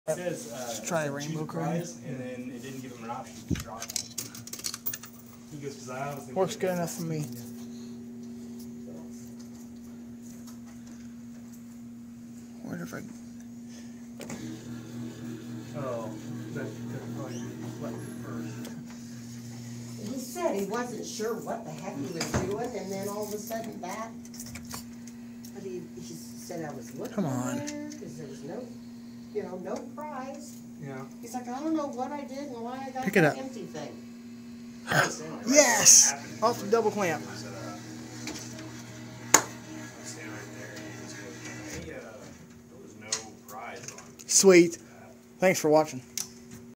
(RAINBOW CRANE) ANIMAL HOUSE CLAW MACHINE@ SILVER DOLLAR CLAW DOES DOUBLE DIVER
THIS WAS FILED YESTERDAY EVENING AT MY LOCAL BOWLING ALLEY  NEAR MY LOCAL MALL HERE YOU WILL NOTICE I GET A HALF PRICE   GAME ONLY PUT ONE QUARTER IN AND GOT A PLAY AND U WILL SEE THE CLAW DO WHAT THEY CALL A DOUBLE DIVE